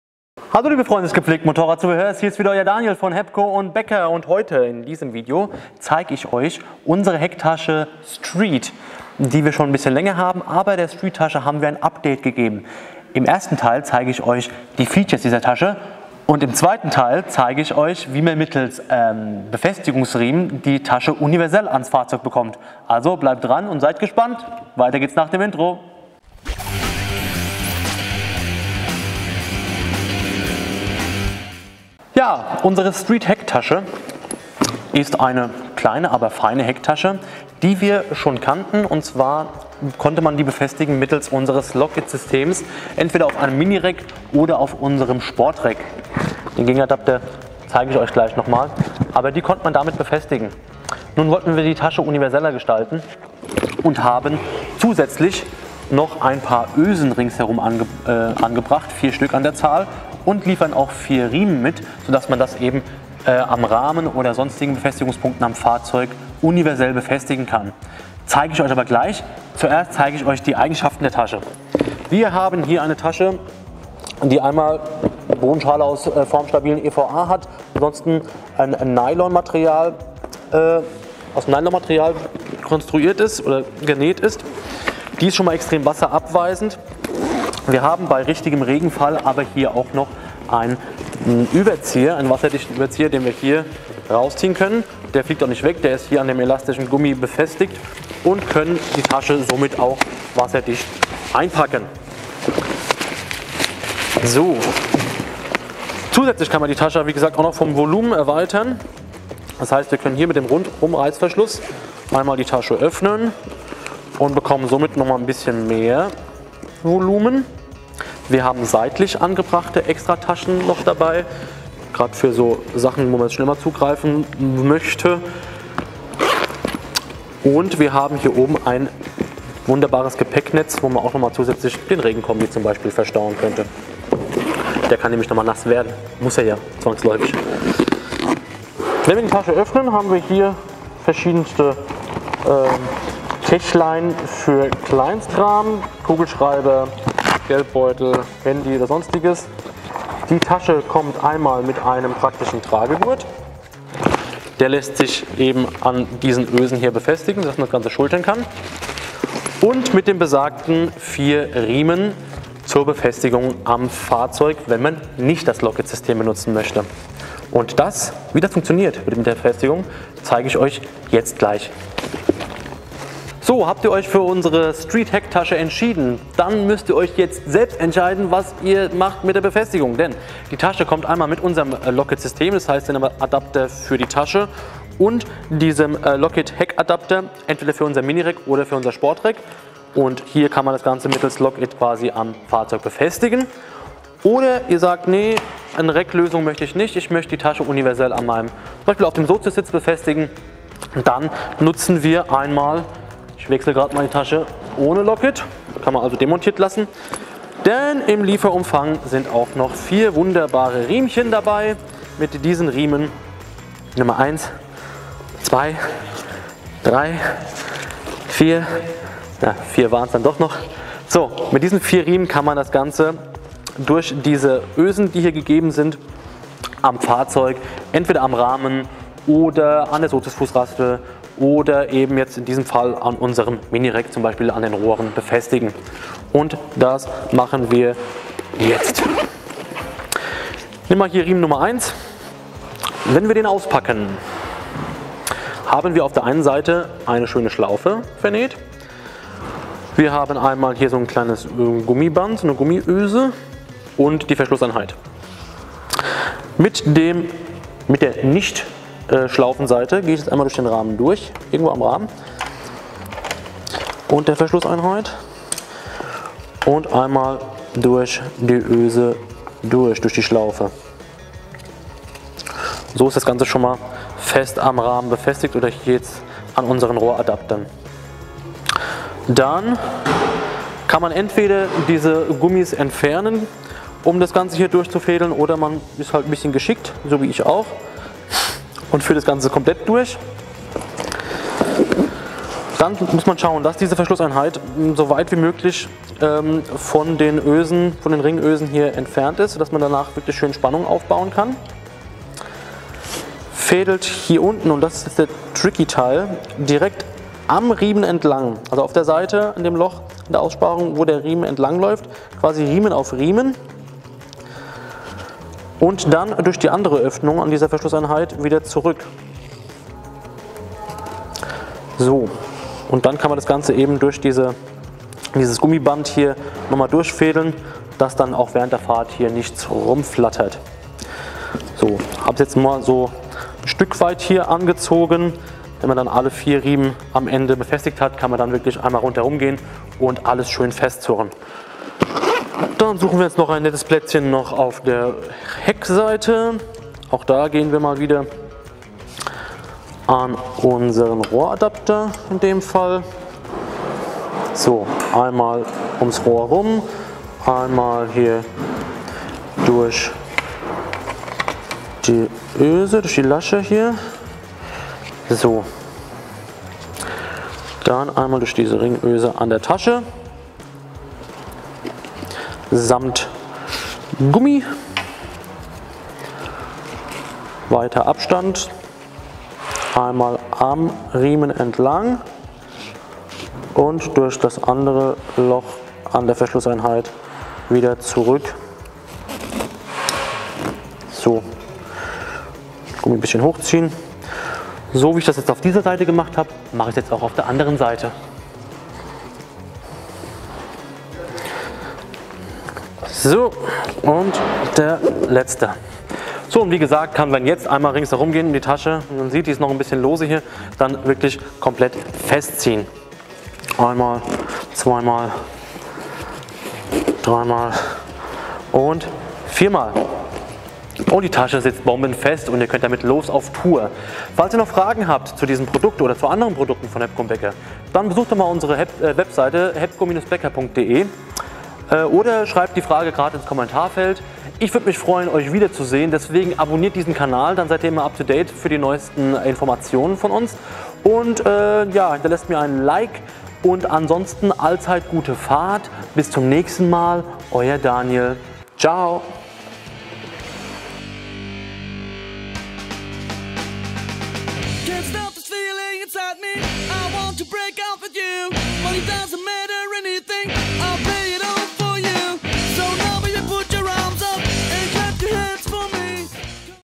Daniel Explains: Street Hecktasche
In diesem Explains-Video erfährst du alles Wichtige über unsere kleine Street Hecktasche und die Befestigungsmöglichkeiten. Schnapp dir einen Kaffee und los gehts! 😎